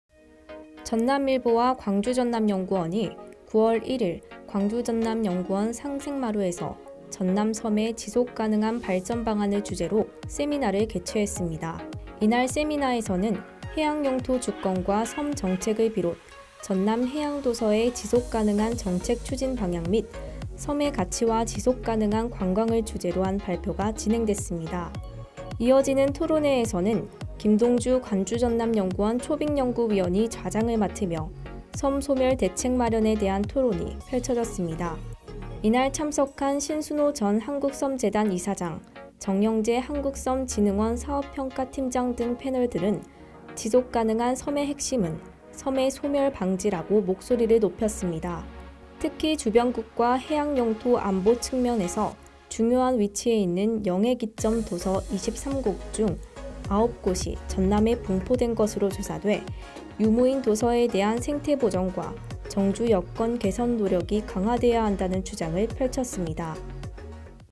전남일보 · 광주전남연구원 ‘전남 섬의 지속 가능한 발전 방안’ 세미나
전남일보와 광주전남연구원이 9월 1일 광주전남연구원 상생마루에서 '전남 섬의 지속 가능한 발전 방안’을 주제로 세미나를 개최했습니다.

이날 세미나에서는 ‘해양영토 주권과 섬 정책’을 비롯 전남 해양도서의 지속 가능한 정책 추진 방향 및 섬의 가치와 지속 가능한 관광을 주제로 한 발표가 진행됐습니다.

이어지는 토론회에서는 김동주 광주전남연구원 초빙연구위원이 좌장을 맡으며 '섬 소멸' 대책 마련에 대한 토론이 펼쳐졌습니다.

이날 참석한 △신순호 전 한국섬재단 이사장 △정영재 한국섬진흥원 사업평가팀장 등 패널들은 지속 가능한 섬의 핵심은 '섬의 소멸' 방지라고 목소리를 높였습니다.

특히 주변국과 해양영토·안보 측면에서 중요한 위치에 있는 영해기점(領海基點) 도서 23곳 중 9곳(39%)이 전남에 분포된 것으로 조사돼 유·무인도서에 대한 생태보전과 정주여건 개선 노력이 강화돼야 한다는 주장을 펼쳤습니다.